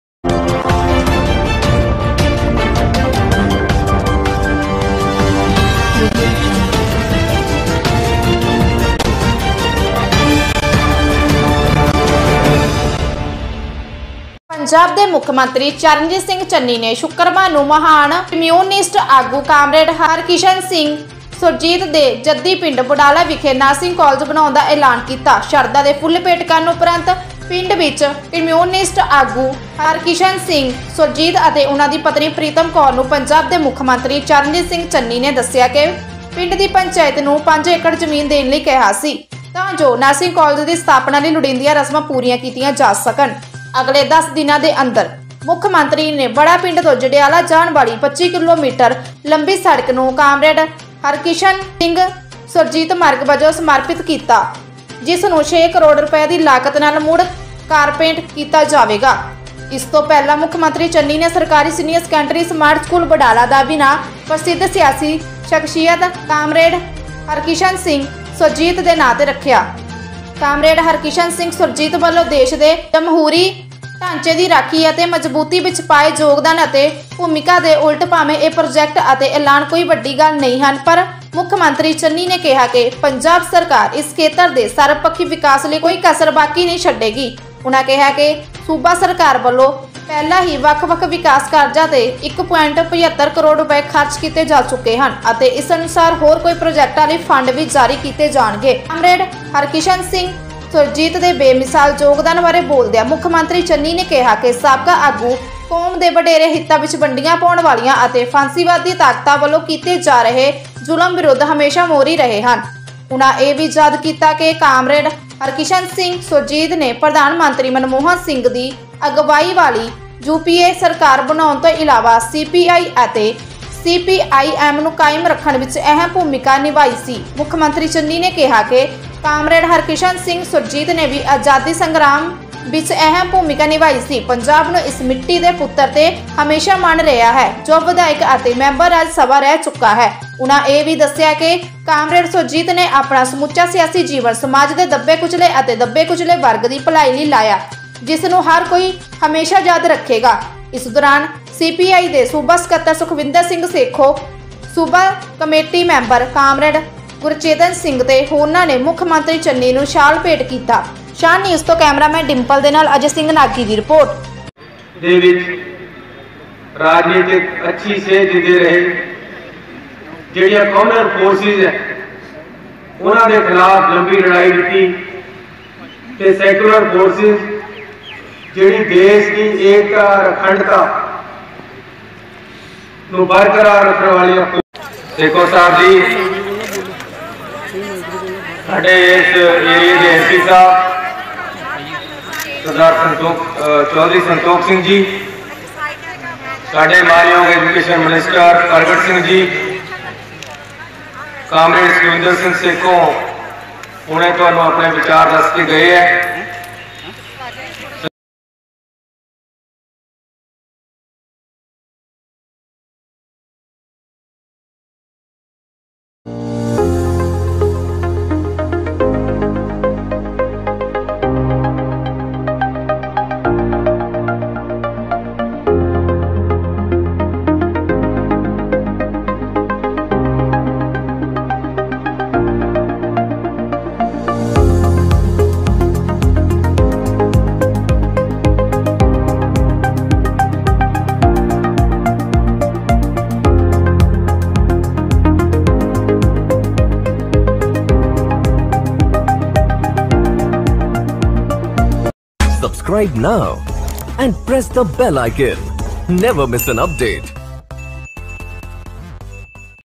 ਮੁੱਖ ਮੰਤਰੀ ਚੰਨੀ ਵੱਲੋਂ ਕਾਮਰੇਡ ਹਰਕਿਸ਼ਨ ਸਿੰਘ ਸੁਰਜੀਤ ਦੇ ਪਿੰਡ ਬੁੰਡਾਲਾ ਲਈ ਨਰਸਿੰਗ ਕਾਲਜ ਦਾ ਐਲਾਨ
ਜ਼ਿਲ੍ਹਾ ਜਲੰਧਰ ਦੇ ਇਤਿਹਾਸਕ ਪਿੰਡ ਬੁੰਡਾਲਾ ਮੰਜਕੀ ਵਿਖੇ